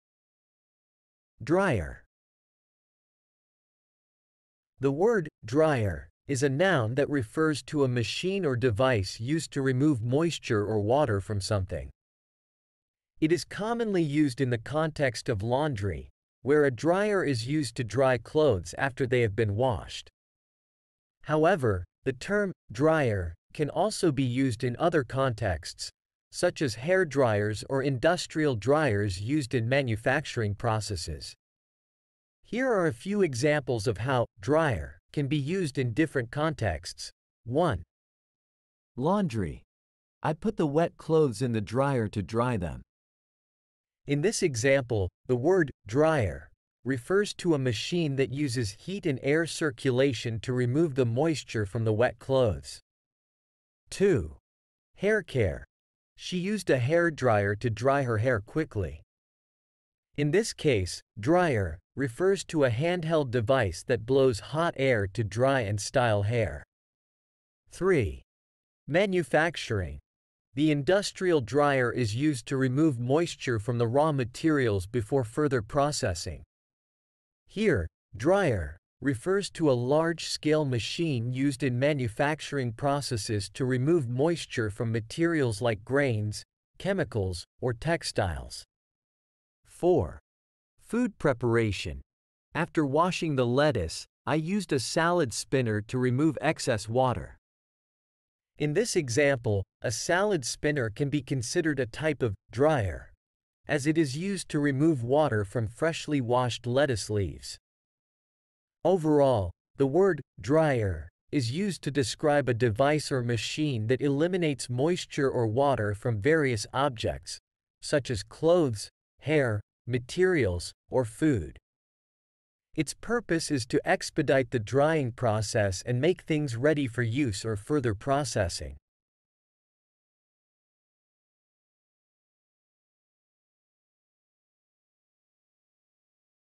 Dryer
The word "dryer" is a noun that refers to a machine or device used to remove moisture or water from something. It is commonly used in the context of laundry, where a dryer is used to dry clothes after they have been washed. However, the term "dryer" can also be used in other contexts, such as hair dryers or industrial dryers used in manufacturing processes.
Here are a few examples of how "dryer" can be used in different contexts:
1. Laundry: "I put the wet clothes in the dryer to dry them." In this example, the word "dryer" refers to a machine that uses heat and air circulation to remove the moisture from the wet clothes.
2. Hair care: "She used a hair dryer to dry her hair quickly." In this case, "dryer" refers to a handheld device that blows hot air to dry and style hair.
3. Manufacturing: "The industrial dryer is used to remove moisture from the raw materials before further processing." Here, "dryer" refers to a large-scale machine used in manufacturing processes to remove moisture from materials like grains, chemicals, or textiles.
4. Food preparation: "After washing the lettuce, I used a salad spinner to remove excess water." In this example, a salad spinner can be considered a type of "dryer" as it is used to remove water from freshly washed lettuce leaves.
Overall, the word "dryer" is used to describe a device or machine that eliminates moisture or water from various objects, such as clothes, hair, materials, or food. Its purpose is to expedite the drying process and make things ready for use or further processing.